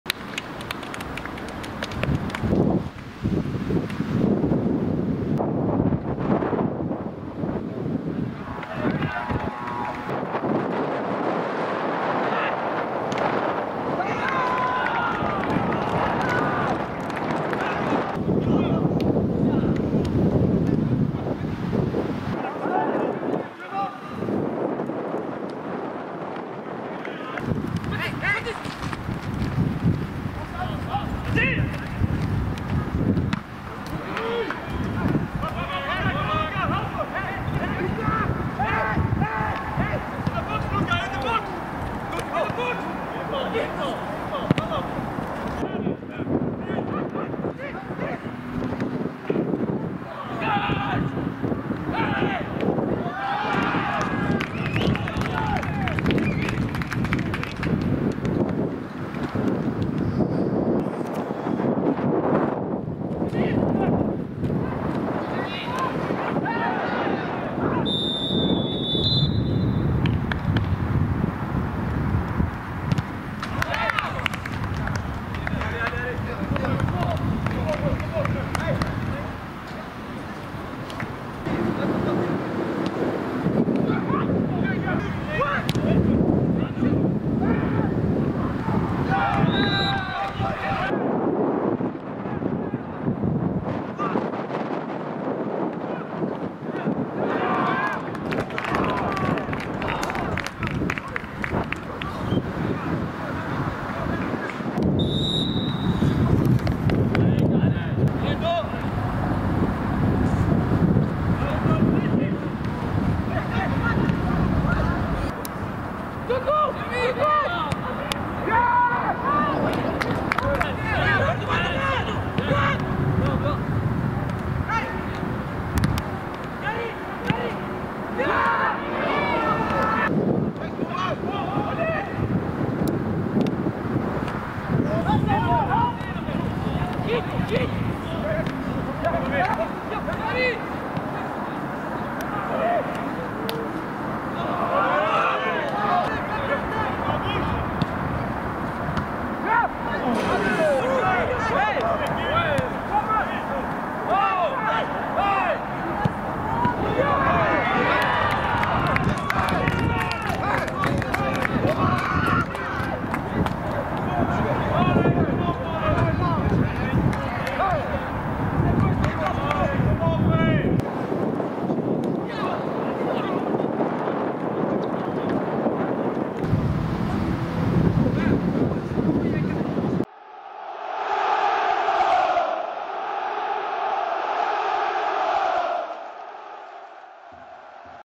Reserves 1A: RSCA 3-2 Cercle Brugge
⚽️ Goals: 4’ Lucas Lissens (1-0), 23’ Olivier Deman (1-1), 27’ Luka Adzic (2-1, pen), 45’ Jonathan Farias (2-2) & 89’ Mathis Suray (3-2)

RSCA : Ilias Moutha-Sebtaoui, Mathis Suray, Mardochee Nzita, Mohamed Loua (24’ Mohamed Bouchouari), Alan Mayanga (61’ Jeremy Doku), Hotman El Kababri, Mohammed Dauda (45’ Olivier Dhauholou), Luka Adzic (61’ Yari Verschaeren), Jakub Kiwior, Lucas Lissens & Théo Leoni